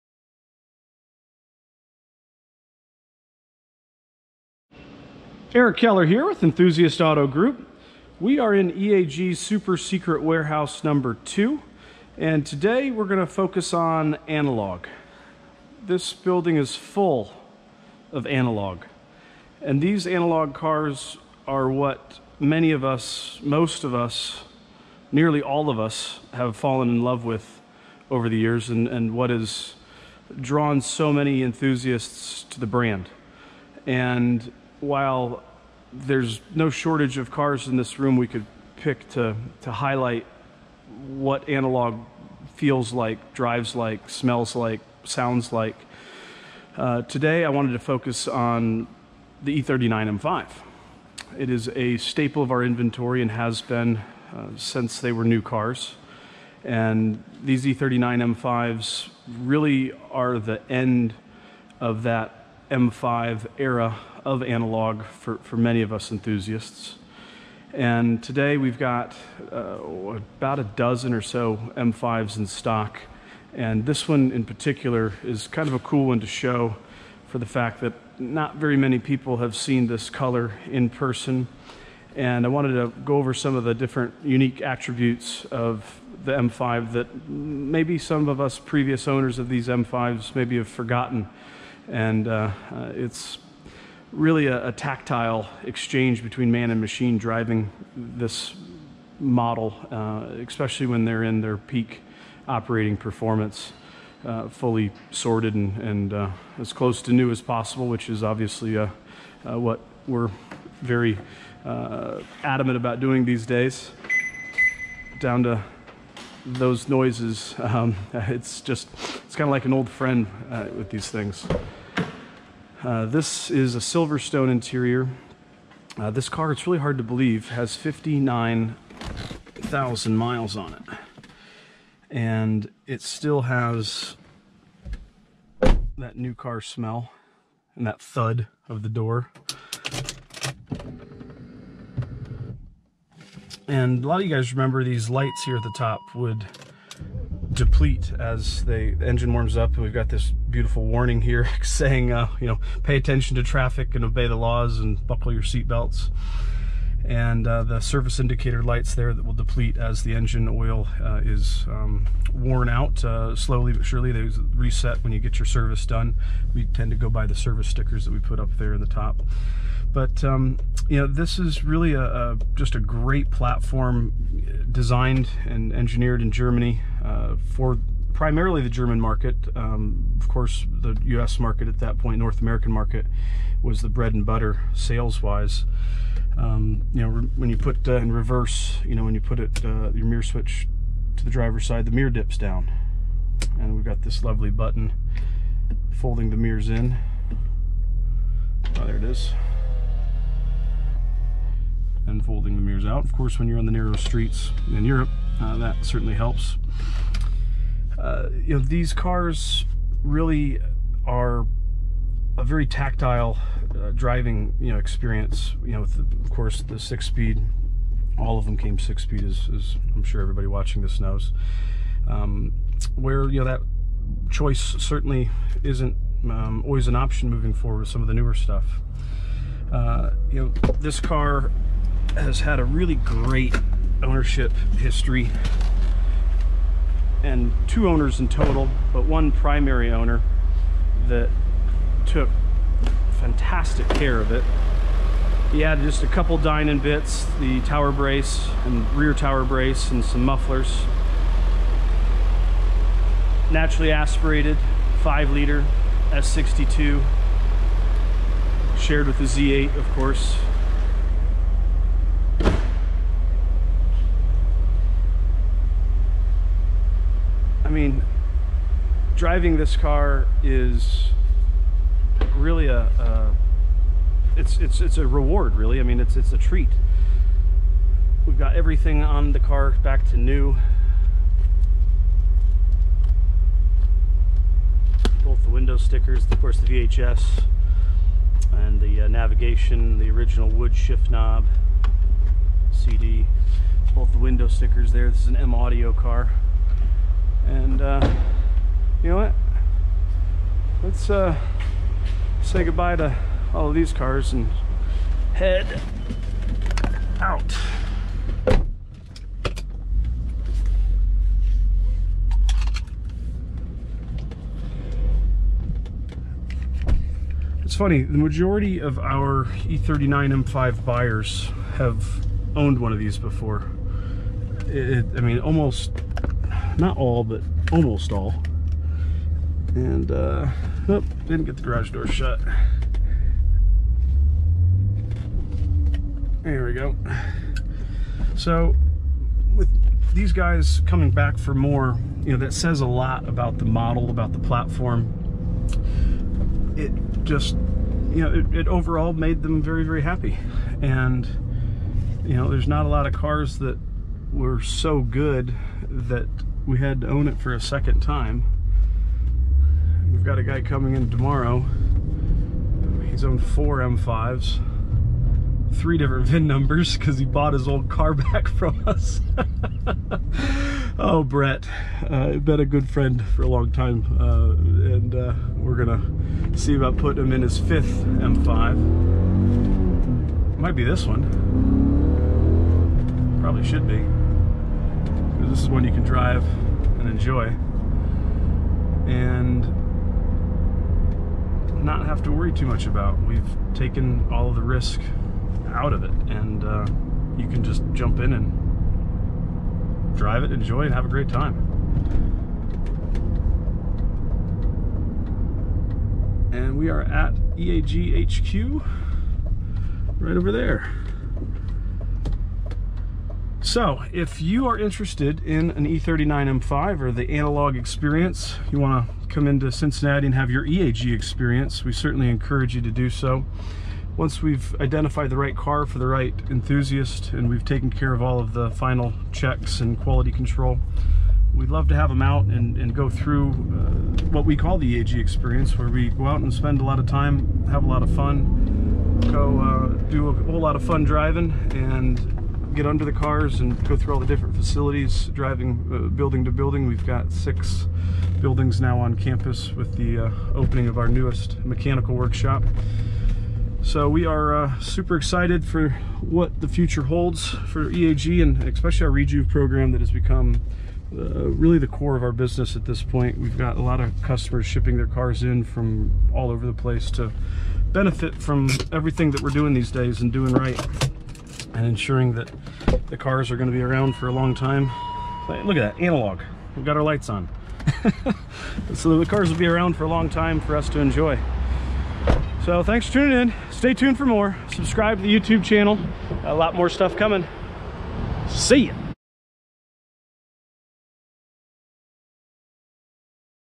The Analog BMW M5 - End of an Era
Which cars in our warehouse would you like to see more of?

We take a stroll through EAG's Super Secret Warehouse #2 and focus our attention on a special 2003 Chiarettorot M5.  This is the last M5 produced with a dipstick to check your oil and when the 6-speed manual was the only transmission option offered.

Enthusiast Auto Group LLC
11608 Reading Road
Cincinnati, Ohio 45241

Facebook and Instagram: @EAGBMW